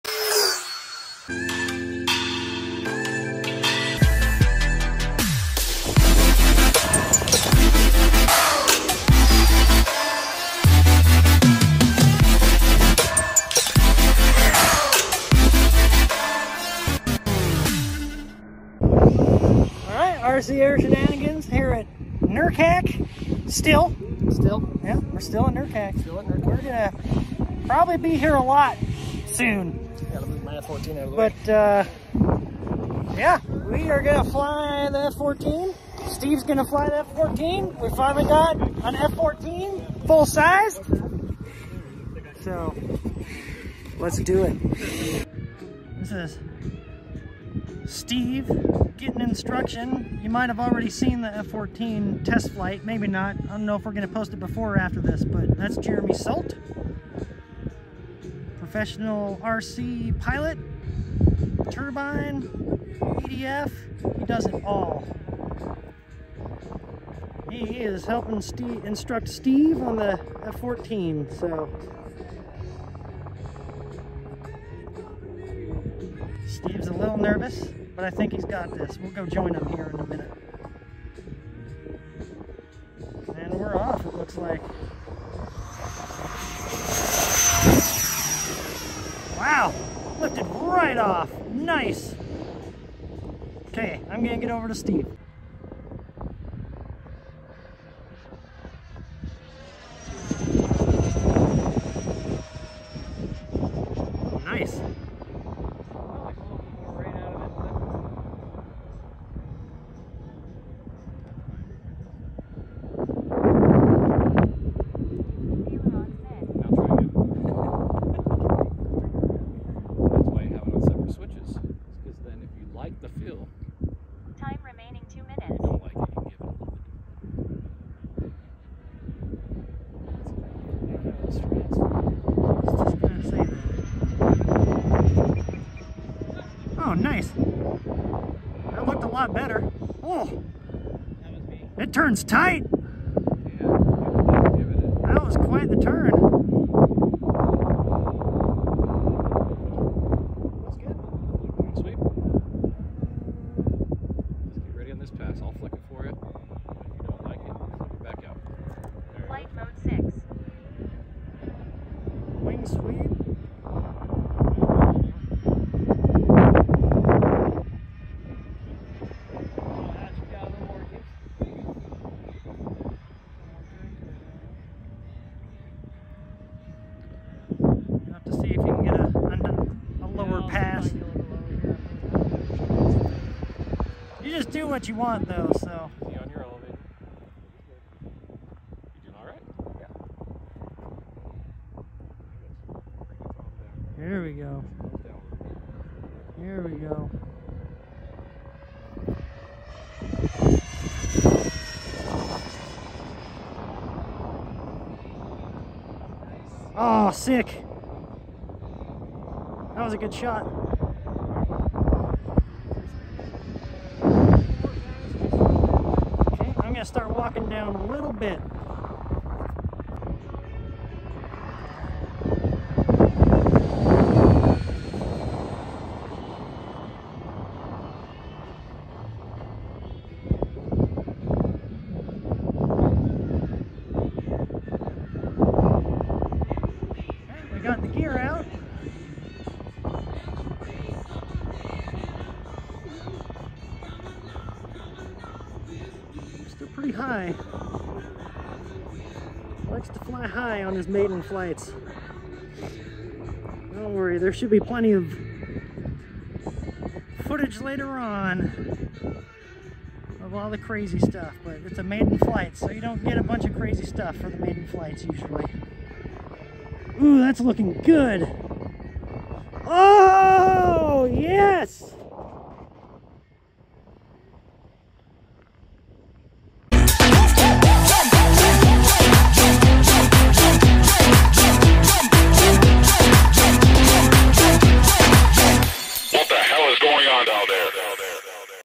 Steve’s maiden Flight of the Freewing F-14
This F-14 has a few modifications-
The power system is the twin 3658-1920Kv brushless inrunner motors and 100A ESCs from the Freewing MiG-29. It also has a modified battery tray- the batteries are no longer stacked, and it has gear upgrades.

We have an Amazon Storefront!
Check out our idea lists for Beginner- advanced planes, gear, tools, and accessories.